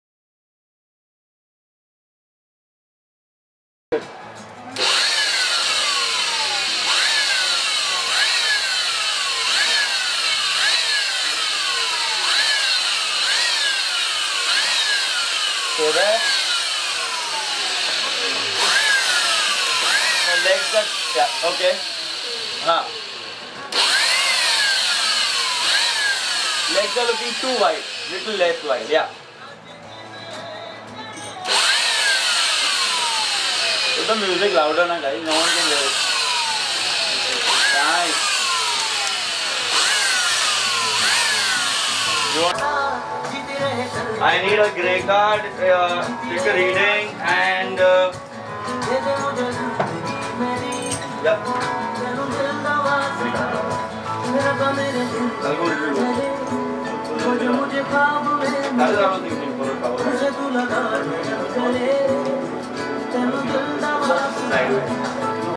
Amrita Rao revealing her hot toned legs at the FHM March Cover Shoot
Amrita Rao in her hottest HOTTEST photo shoot till date for FHM INDIA MAGAZINE.